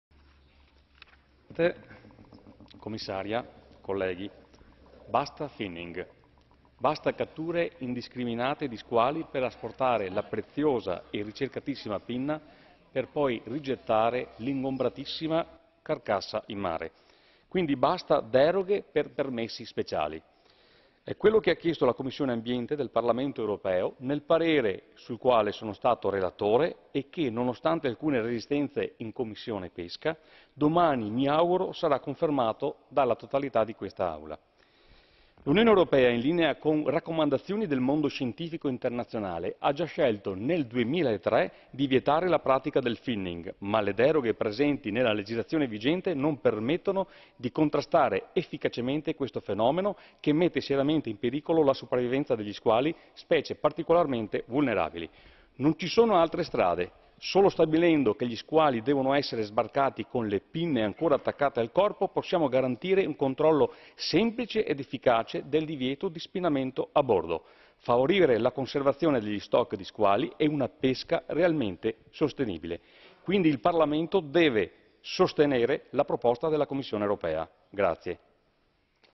Andrea Zanoni on Conservation of fishery resources through technical measures
Speech by Andrea Zanoni MEP (ALDE ADLE) on :
Conservation of fishery resources through technical measures for the protection of juveniles of marine organisms - Baltic salmon stock and the fisheries exploiting that stock - Small-scale and artisanal fisheries and CFP reform - External dimension of the common fisheries policy - Removal of fins of sharks on board vessels

[MEP Speech] [Language IT original]

Zanoni-OR_121121_215957_mesp_it